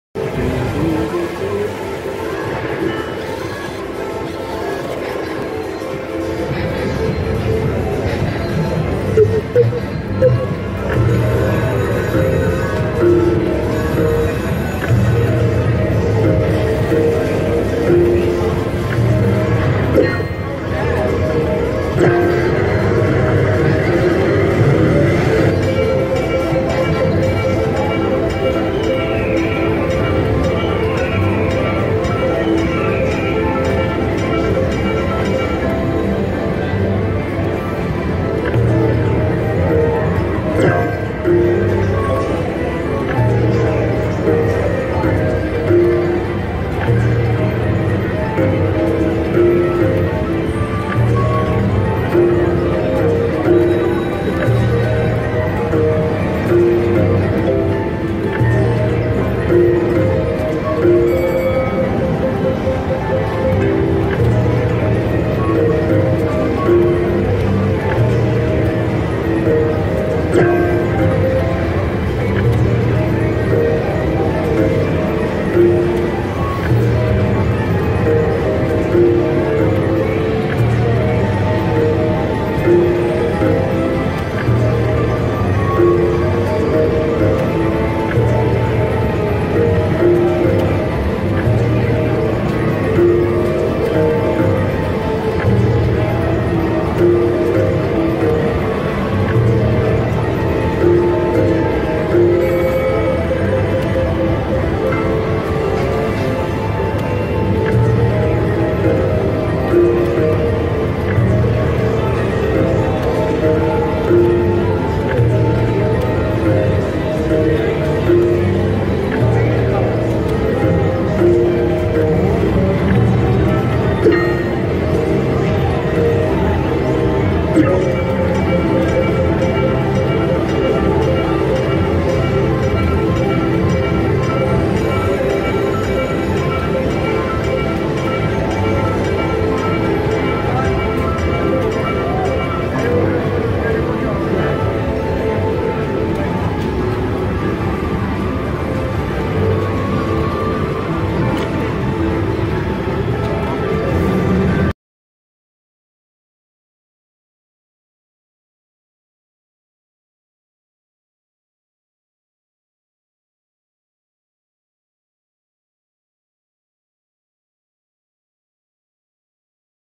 Red Screens 💥 Live Play Slot Machines
💥 From $21 to $170 On Slot Machine Hard Rock Casino Hollywood
I went in with only $40 on me. Lost $19 on my first choice, then tried this new machine I had not seen before.